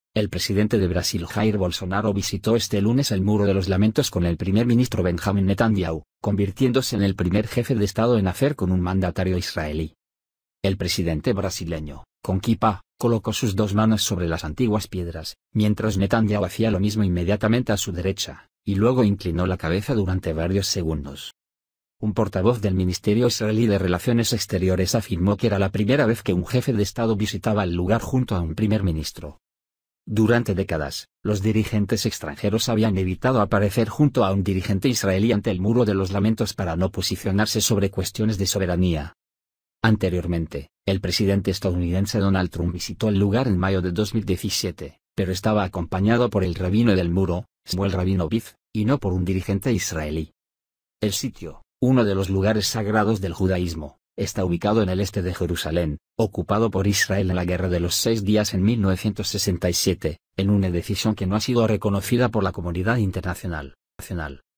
Bolsonaro hace historia en Israel
El presidente de Brasil Jair Bolsonaro visitó este lunes el Muro de los Lamentos con el primer ministro Benjamin Netanyahu, convirtiéndose en el primer jefe de Estado en hacerlo con un mandatario israelí.

El presidente brasileño, con kipá, colocó sus dos manos sobre las antiguas piedras, mientras Netanyahu hacía lo mismo inmediatamente a su derecha, y luego inclinó la cabeza durante varios segundos.

Un portavoz del ministerio israelí de Relaciones Exteriores afirmó que era la primera vez que un jefe de Estado visitaba el lugar junto a un primer ministro.

Durante décadas, los dirigentes extranjeros habían evitado aparecer junto a un dirigente israelí ante el Muro de los Lamentos para no posicionarse sobre cuestiones de soberanía.

Anteriormente, el presidente estadounidense Donald Trump visitó el lugar en mayo de 2017, pero estaba acompañado por el rabino del Muro, Shmuel Rabinovitz, y no por un dirigente israelí.
El sitio, uno de los lugares sagrados del judaísmo, está ubicado en el este de Jerusalén, ocupado por Israel en la Guerra de los Seis Días en 1967, en una decisión que no ha sido reconocida por la comunidad internacional.